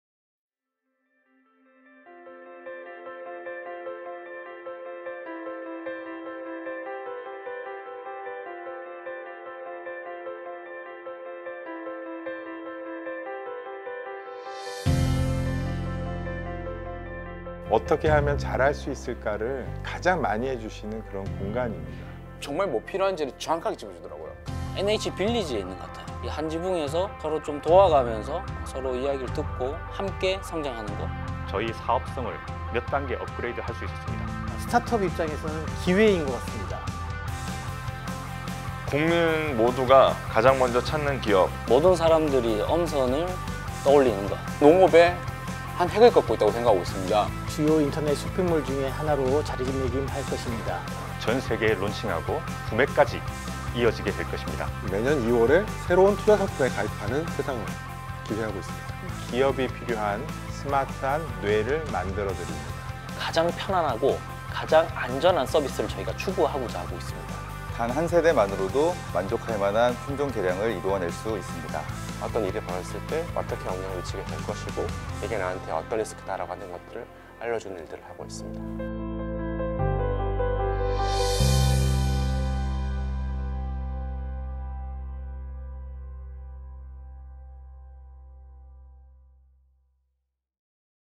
[NH농협은행/NH디지털혁신캠퍼스] NHDCP 2기 데모데이 스케치 영상
NHECP 2기 데모데이의 생생한 현장을 공개 해협!

디지털 금융혁신을 선도하고 있는 농협은행과
임팩트 밴처 투자 및 육성 전문 기업 크래비스파트너스가 함께한
데모데이의 현장 다 함께 느껴보실까협?